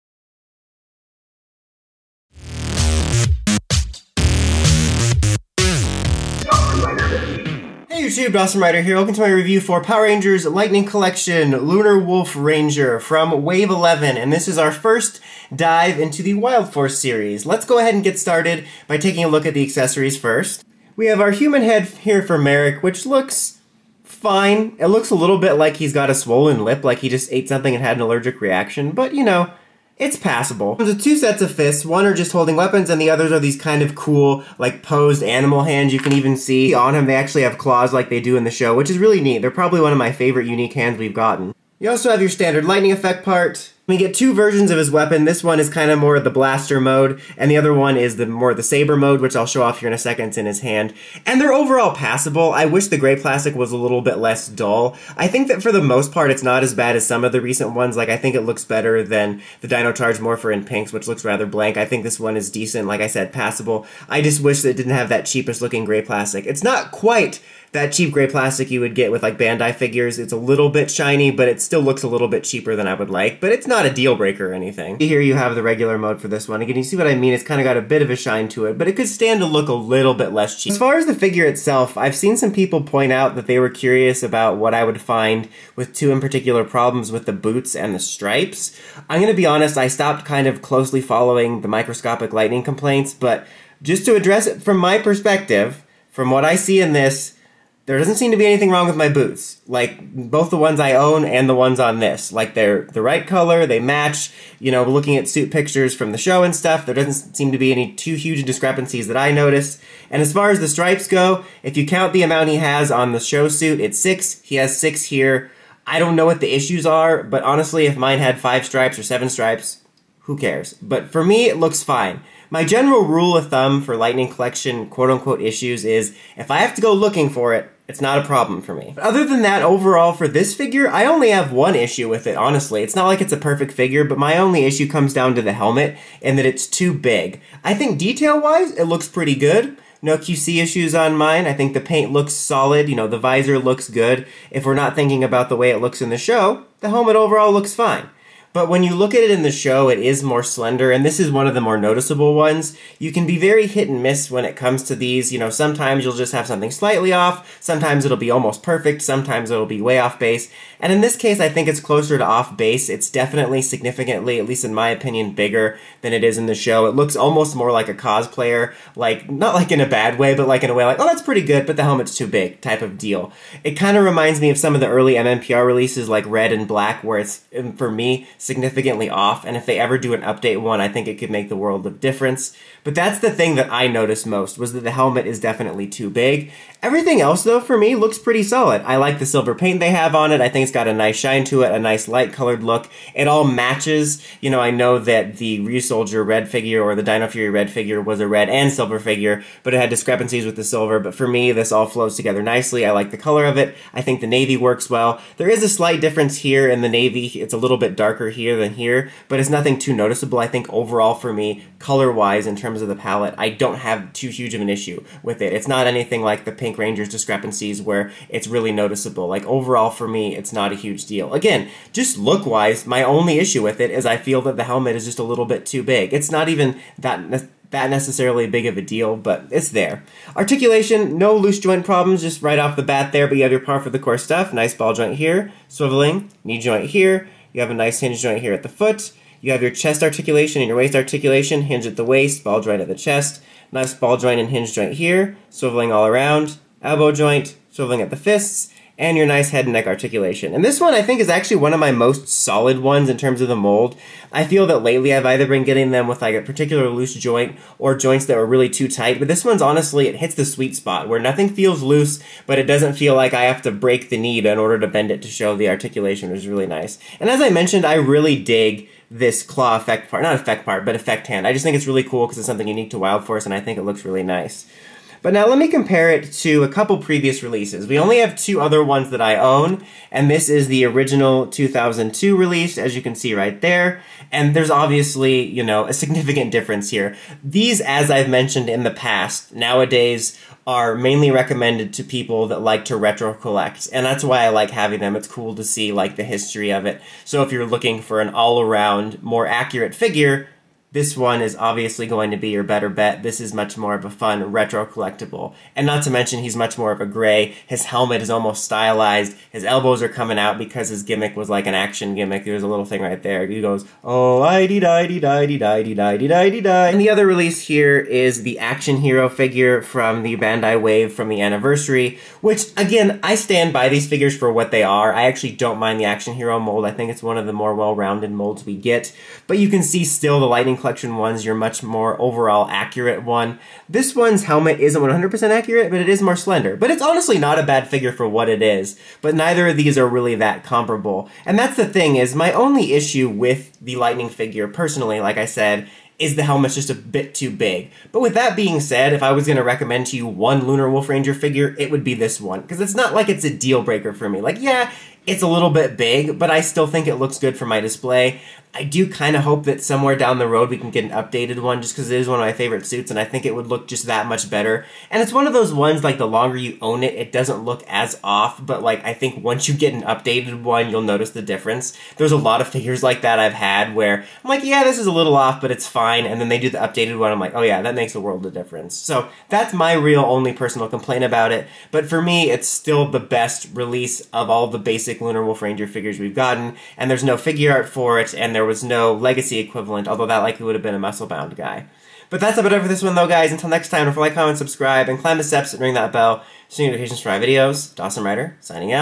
Power Rangers Lightning Collection Wild Force Lunar Wolf Ranger Figure Review
Welcome to my review of the Lunar Wolf Wild Force Ranger figure from the Hasbro Power Rangers Lightning Collection Wave 11. This is our first release for the Wild Force Series. Does it start of on the right foot? (or boot). I also compare it to the original 2002 Figure and the Bandai Action Hero release.